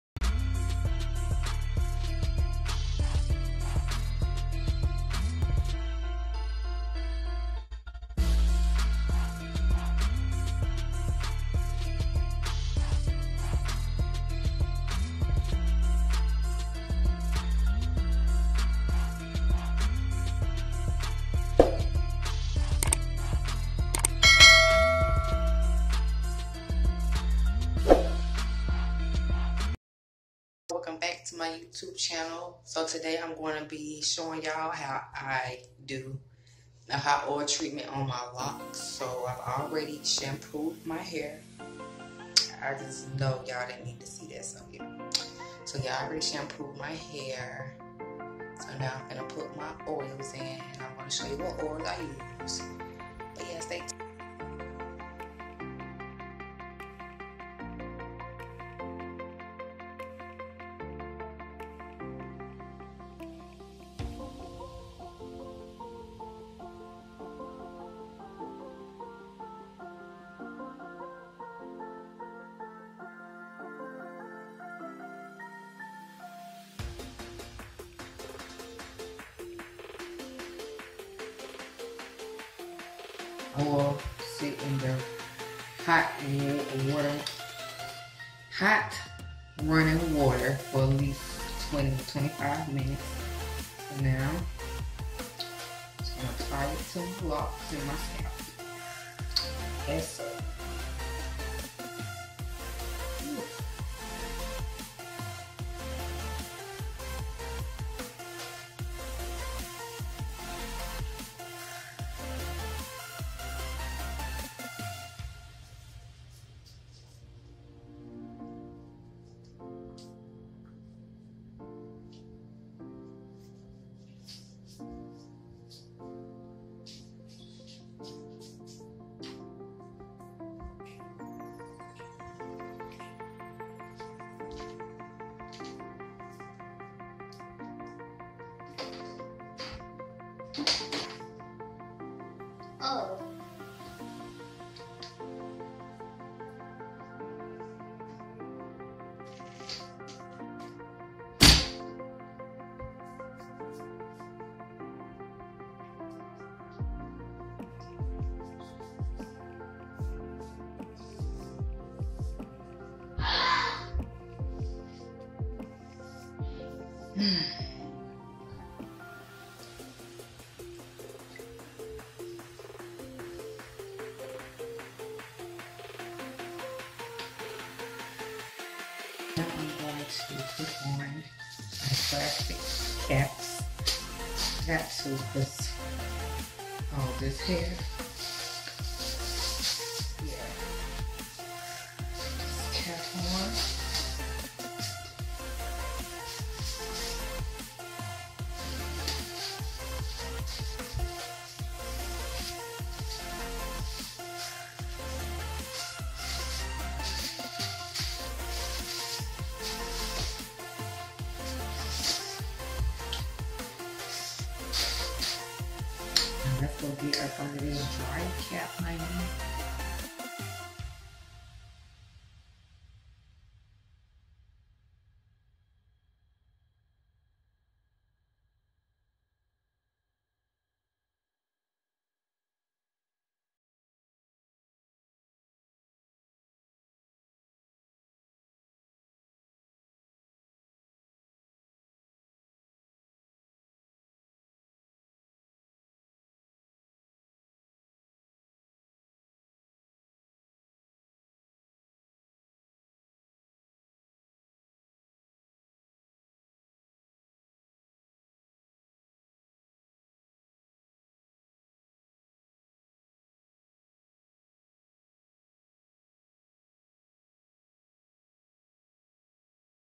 MY EASY HOT OIL TREATMENT METHOD ON LOCS|FT. ROYAL CROWN HAIR GROWTH OIL+STIMULATING SHAMPOO
💫🦄You can place orders through my Instagram pages below